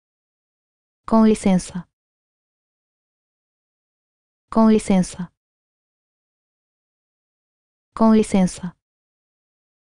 How to say 'Excuse me.' in Portuguese
In this video you will learn how to say 'Excuse me.' in Portuguese - Com licença.. The Learn Portuguese channel helps you improve your language skills and has all the words you need. Listen, learn, and speak Portuguese.